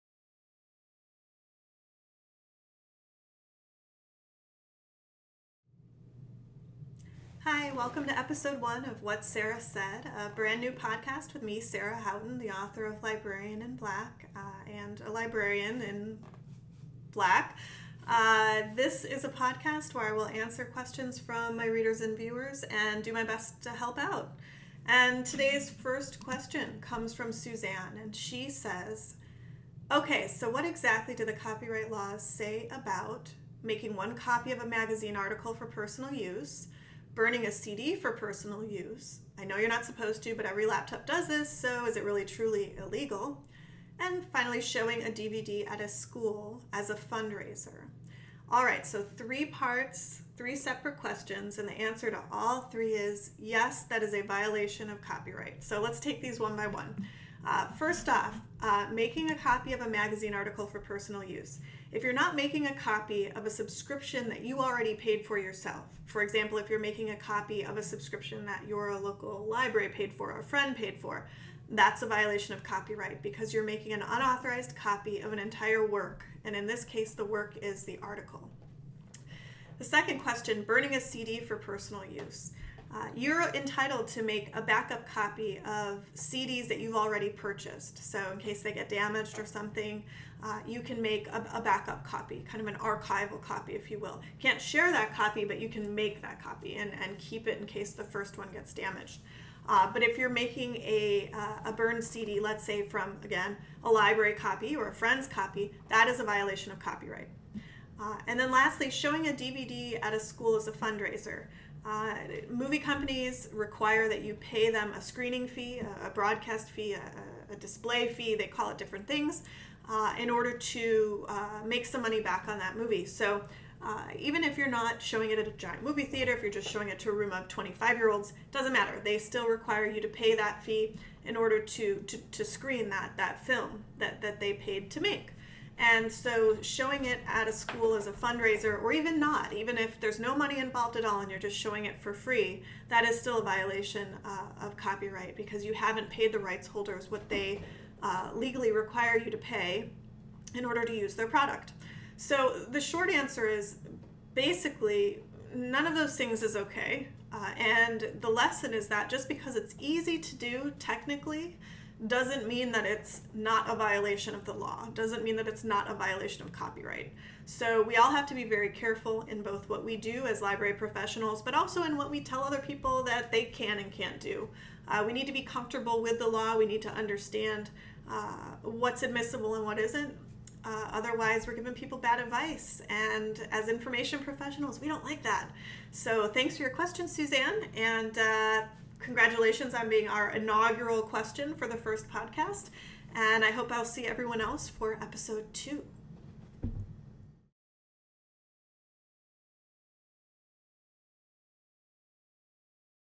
What Sarah Said: Episode 1 - Copyright Trifecta
Suzanne poses some frequently asked copyright questions to Librarian in Black, Sarah Houghton.